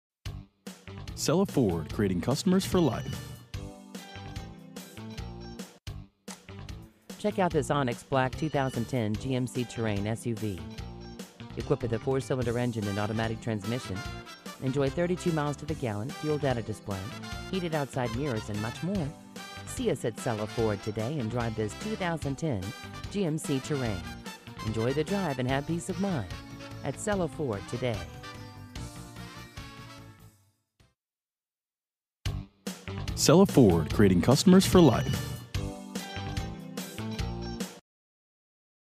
2010 GMC Terrain Cella Ford New Bern, NC 28561
GMC Terrain Imagine driving this Onyx Black Gmc Terrain SUV, equipped with a 4 Cylinder engine and an automatic transmission. Enjoy an exceptional 32 miles to the gallon on this great SUV with features like Heated Driver and Passenger Seating, Privacy Glass, Fuel Data Display, Heated Outside Mirrors, Power Driver's Seat, Back Up Camera, Tire Pressure Monitoring System, Ambient Light Package, Remote Ignition System, Auto-Dimming Mirrors, on steering wheel audio and cruise controls, and much more. Enjoy the drive and have peace of mind in this Gmc Terrain. See us at Cella Ford today!
MILES: 39,829
VIN:2CTALFEW1A6333610